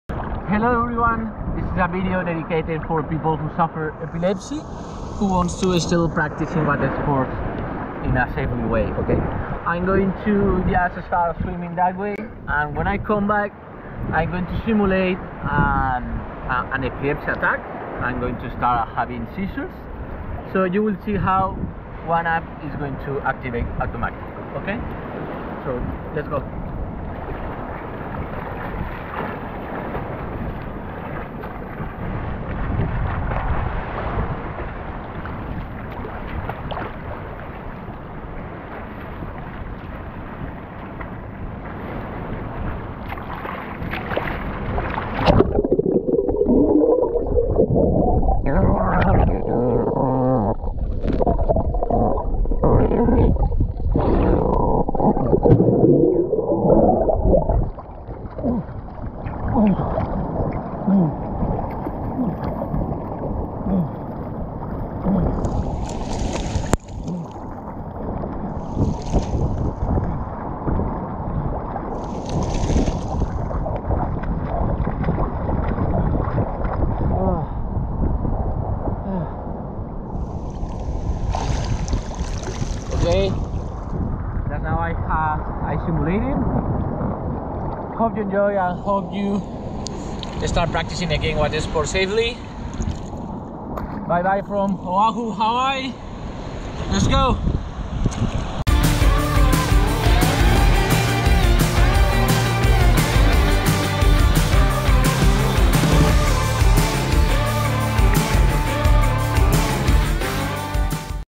Wuanap activation by Epilepsy attack
We show a simulated epilepsy attack, where Wuanap is going to activate in less than 5 seconds to prevent drowning the person who started having seizures. Wuanap will notice this extreme problem to the user, and it will inflate automatically to maintain the user's airways outside the water.